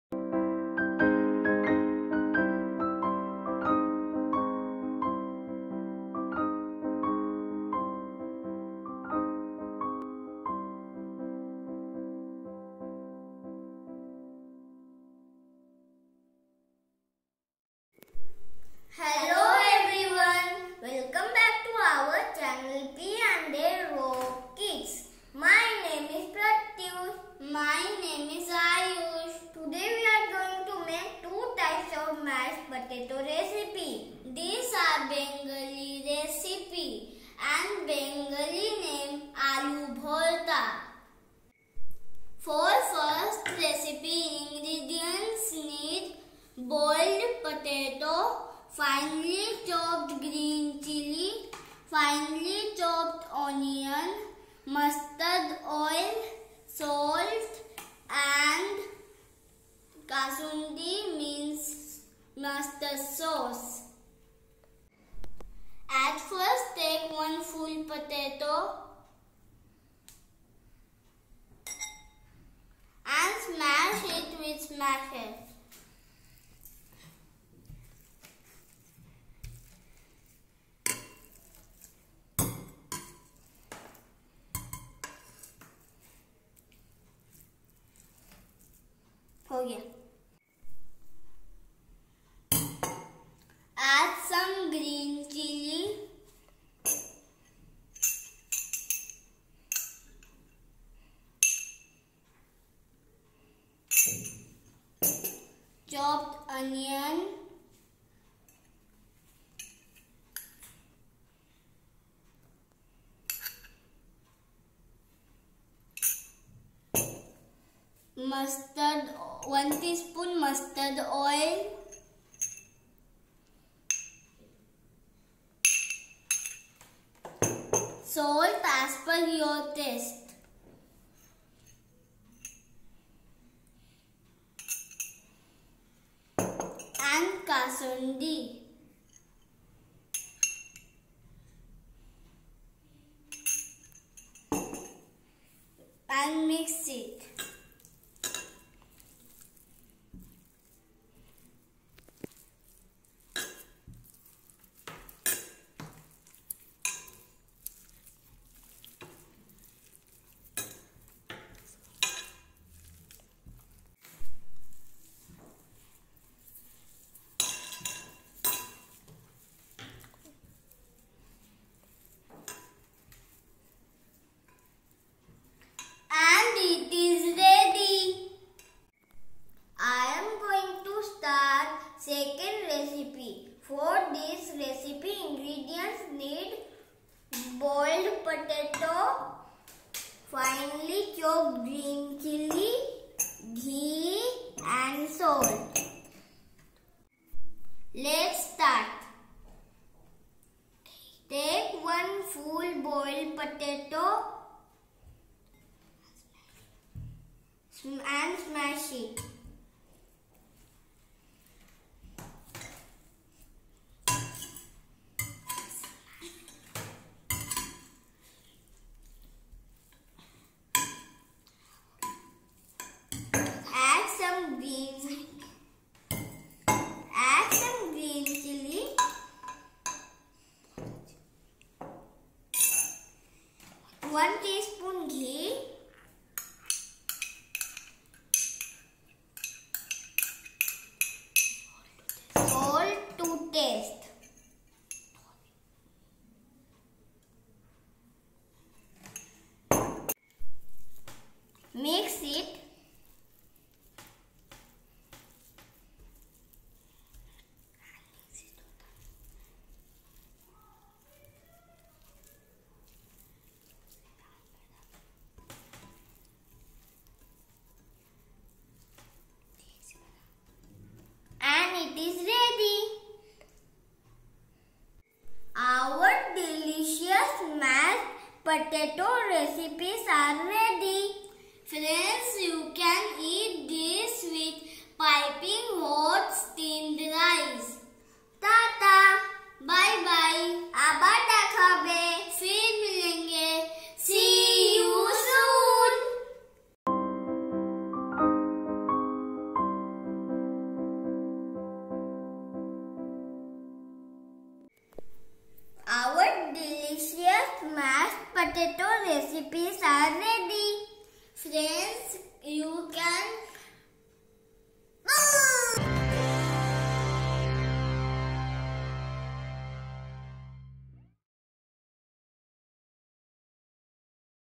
Bengali Style Mashed Potato Recipes | Aloo Bharta Recipes
** In this video we made bengali style mashed potato. Its bengali name is aloo bharta.